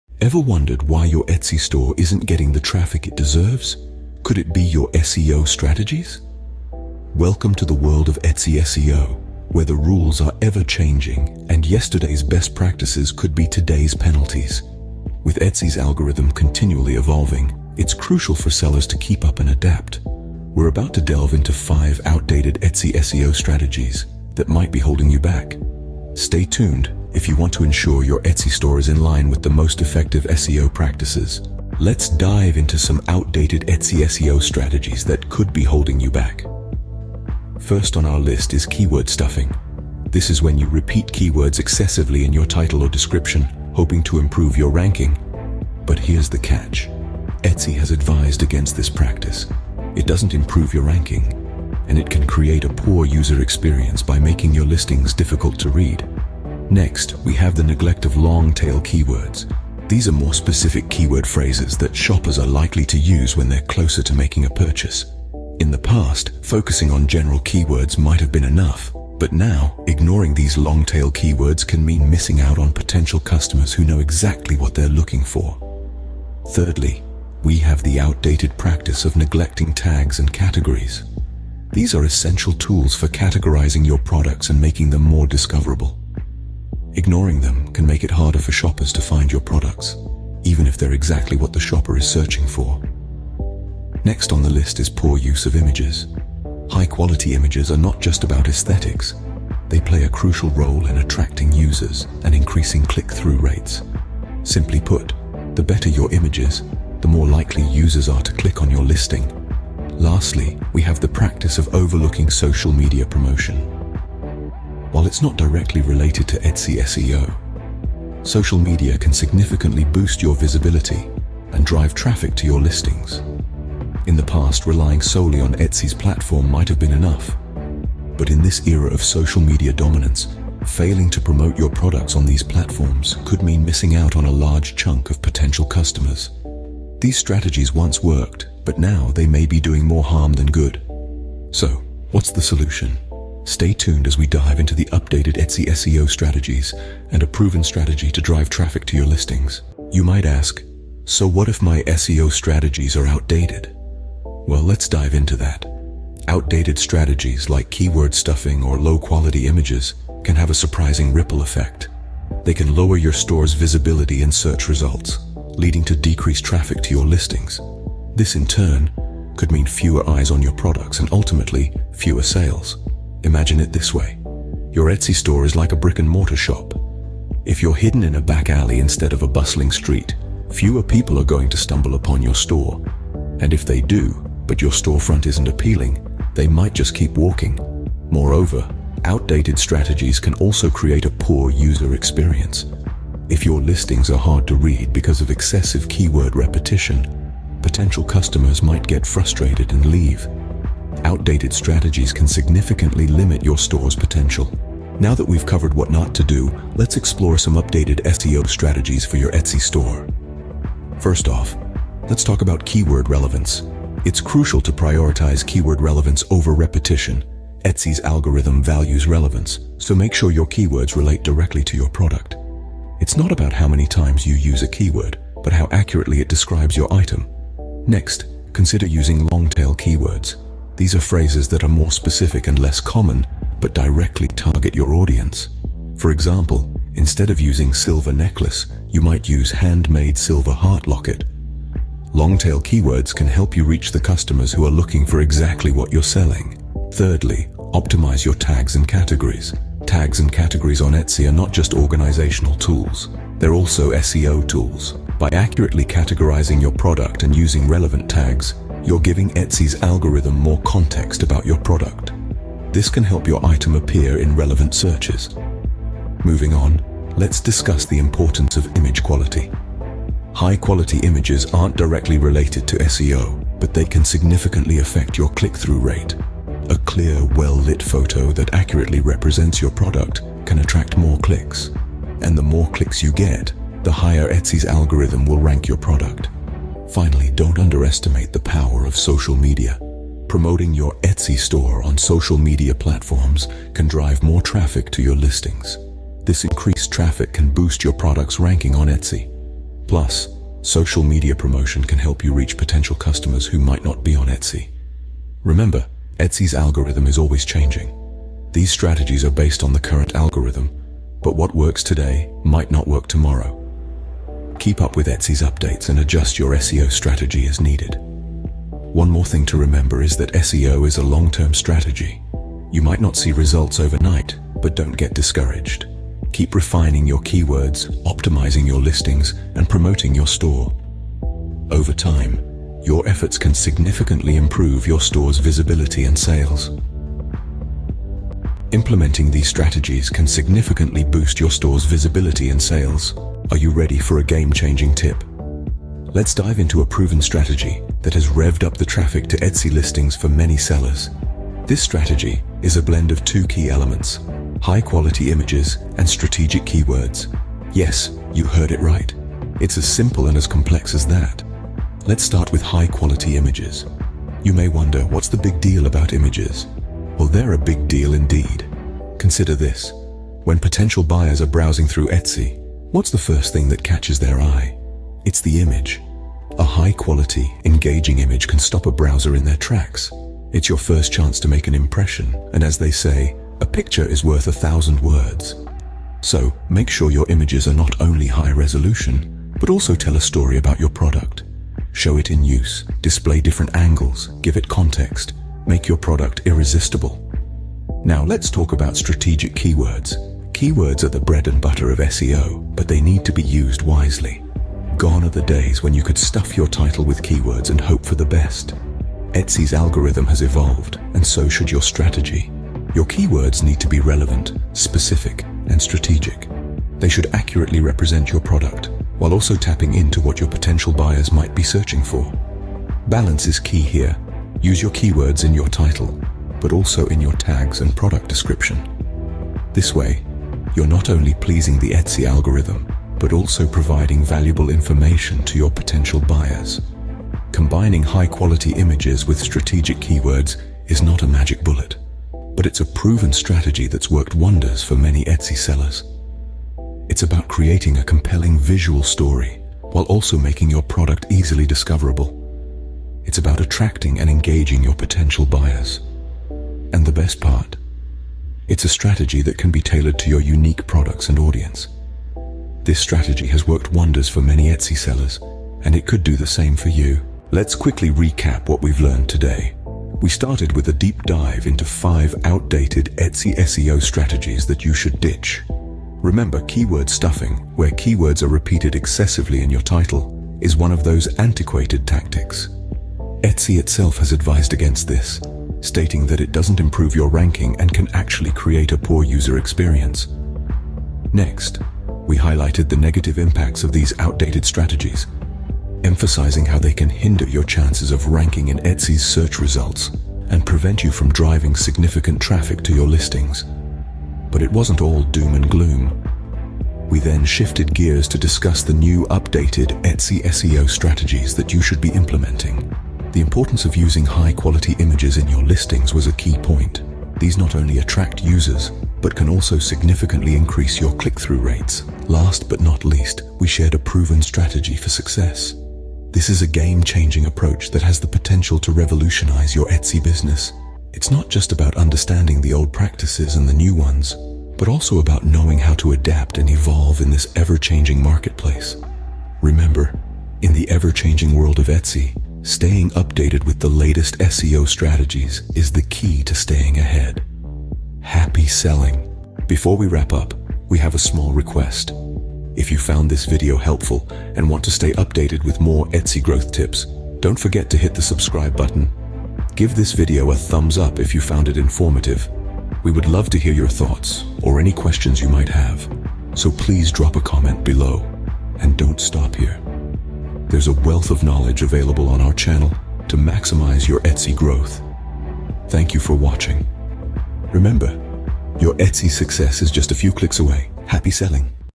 Ditch & Switch: Outdated Etsy SEO Strategies Revisited
Are you an Etsy seller looking to level up your SEO game? If so, look no further!

In this video, we dive deep into the world of Outdated Etsy SEO, exposing the techniques that no longer work and sharing the essential knowledge you need to stay ahead.

Join us as we pull back the curtain on outdated practices and unveil the secrets to optimizing your Etsy listings for maximum visibility and sales.

From outdated keyword stuffing to ineffective tagging strategies, we cover it all, ensuring you have the latest SEO insights at your fingertips.

– Watch now and unlock the true potential of your Etsy store!

OUTLINE:
00:00:00
Introduction

00:00:31
Outdated Etsy SEO Strategies

00:02:29
The Impact of Outdated Strategies

00:03:24
Updated Etsy SEO Strategies

00:05:43
A Proven Strategy

00:07:47

00:09:09

00:09:41
End Sting

In this video, we're going to revisit outdated Etsy SEO strategies and show you how to ditch them for more effective ones.

Etsy is a great place to sell your handmade crafts, and with over 10 million active buyers, it's a powerful platform. However, many Etsy sellers are using outdated SEO strategies that aren't working anymore. In this video, we're going to show you how to ditch these strategies and adopt more effective techniques that will boost your Etsy traffic and sales!